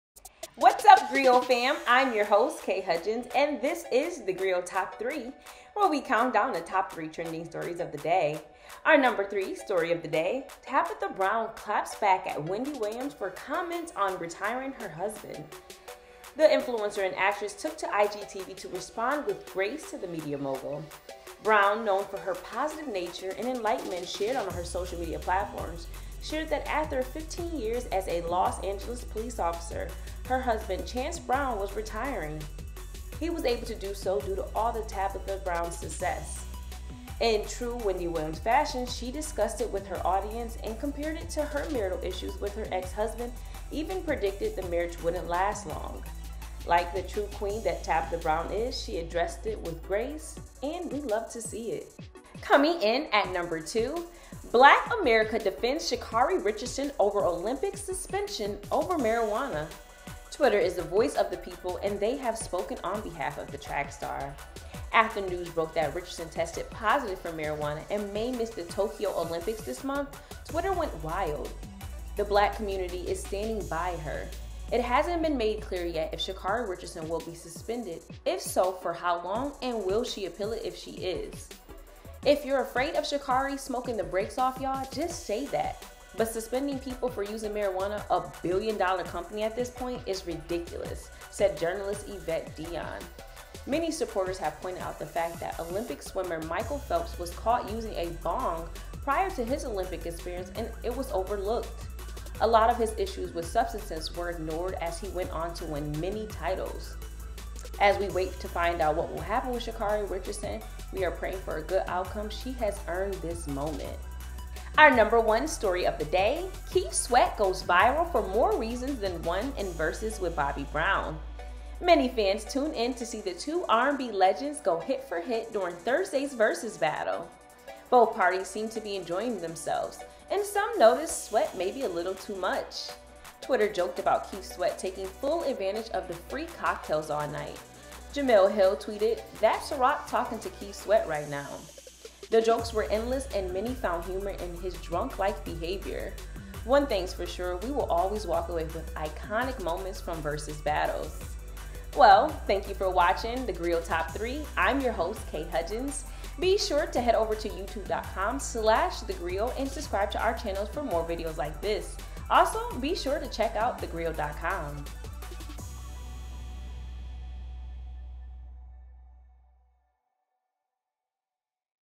Black America defends Sha’Carri Richardson over Olympics suspension for marijuana| Grio Top 3
Welcome to TheGrio Top 3 with Kay Hudgins
-Black America defends Sha’Carri Richardson over Olympics suspension for marijuana
-Tabitha Brown claps back at Wendy Williams for comments on retiring her husband
-Keith Sweat goes viral for more reasons than one in Verzuz with Bobby Brown